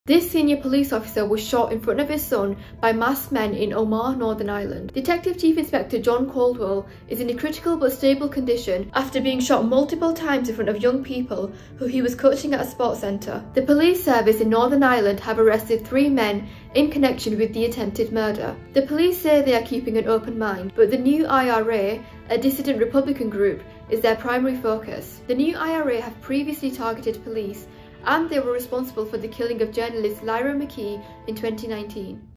New IRA are ‘primary focus’ after detective was shot
A senior off-duty detective was shot multiple times in Omagh and police say the New IRA are their primary focus.